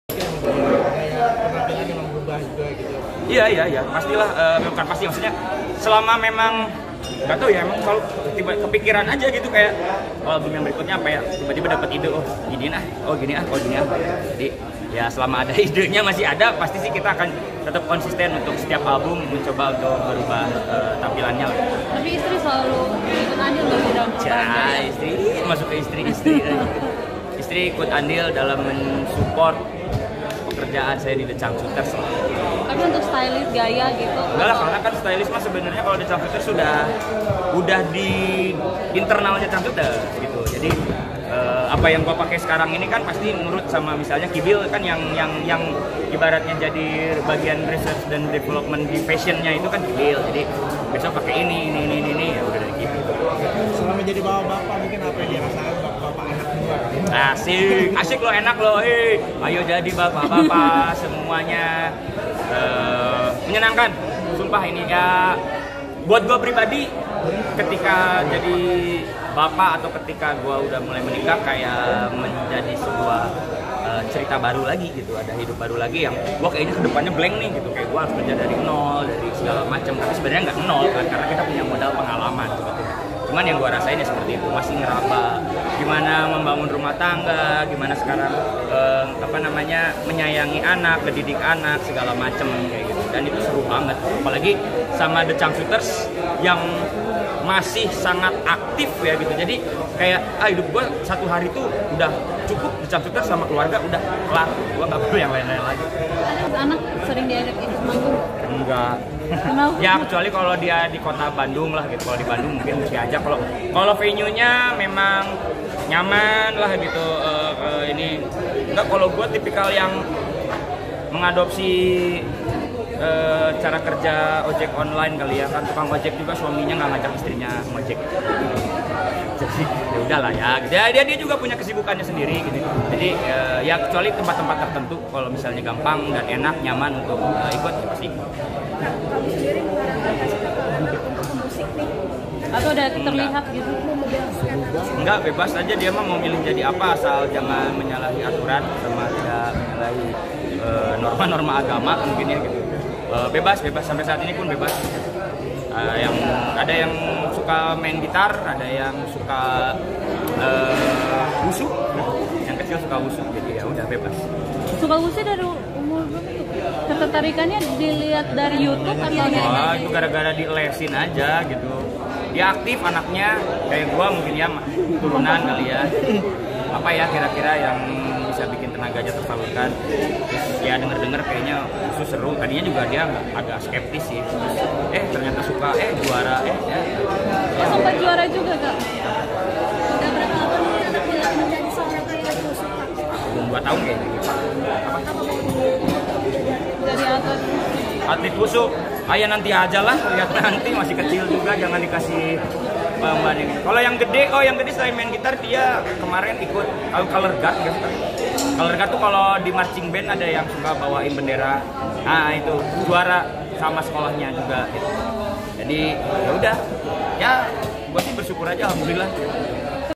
Tria The Changcuters Sedang Menikmati Asiknya Menjadi Ayah
Mohammad Tria Ramadhani, atau yang akrab disapa Tria, vokalis dari grup band The Changcuters, mengungkapkan bahwa ia sedang menikmati perannya sebagai seorang ayah.

Tria saat ini telah memiliki dua anak yang bernama Lenonina Maryam Daria dan Jaggira Hannah Daria.